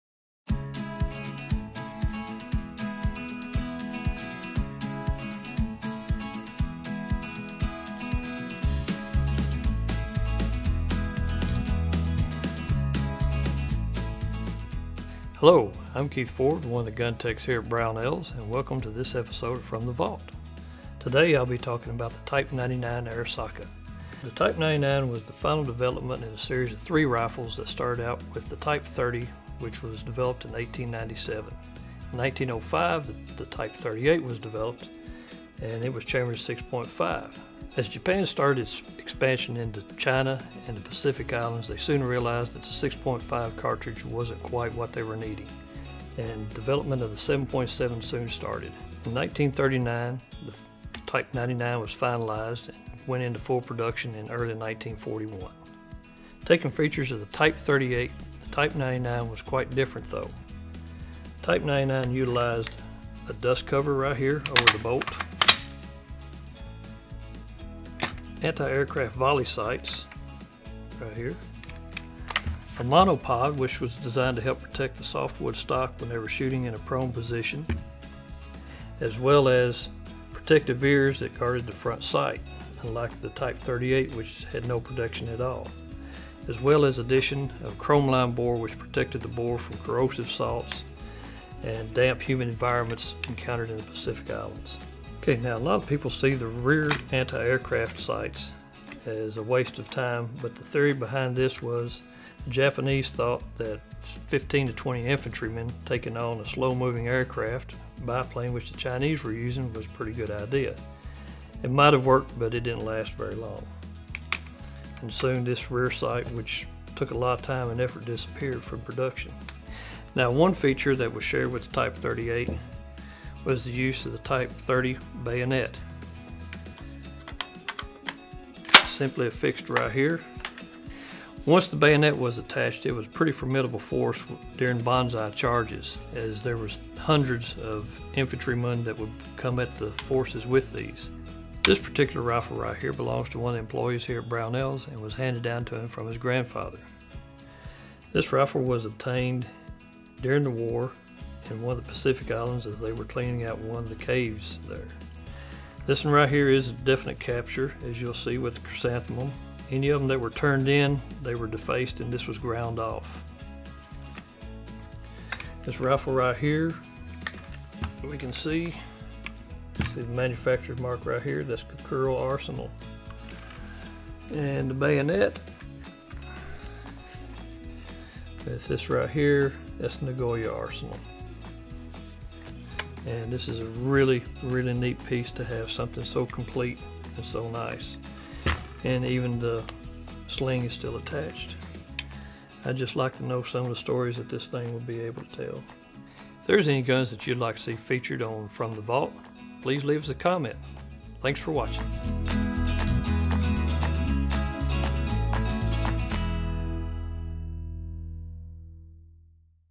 From the Vault: Type 99 Japanese Arisaka
Join Brownells GunTech Keith Ford as he offers an overview of the collectible Type 99 rifle often referred to as the Japanese “Arisaka.” Designed by Colonel Arisaka Nariakira and used by the Imperial Japanese Army during World War II, this bolt-action rifle featured a wire monopod, an anti-aircraft sighting device and was chambered in 7.7x58mm. The Type 99 was produced in four versions, Short, Long, Paratroop and Sniper. Late war “last ditch” rifles are distinguished by their crude manufacturing and poor finishes. If you have a firearm you’d like to see featured in the From the Vault series, please let us know.